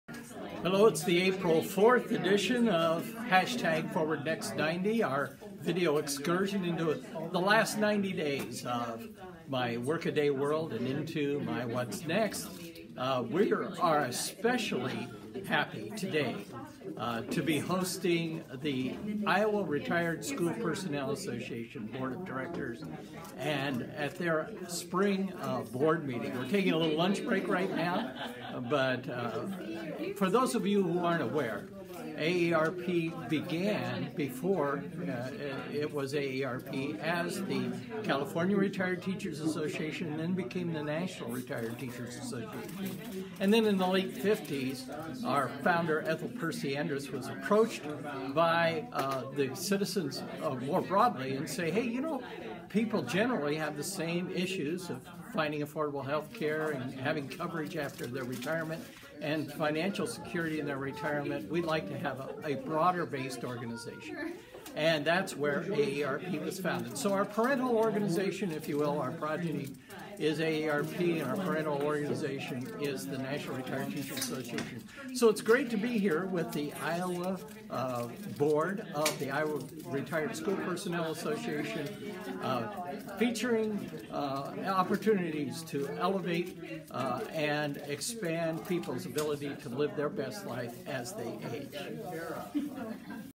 APR 4 edition fwdNext90 - IRSPA Spring Board meeting
Today I am honored to welcome the members of the state board of the Iowa REtired School Personnel Association  to the AARP Iowa offices.  The National Retired Teachers Association (NRTA) is the predecessor of AARP. The NRTA continues to represent the education community within the broader AARP organization.  What a great group of retired professionals. If you are a former educator, look for a local unit of the retired teachers (IRSPA) and get to know them.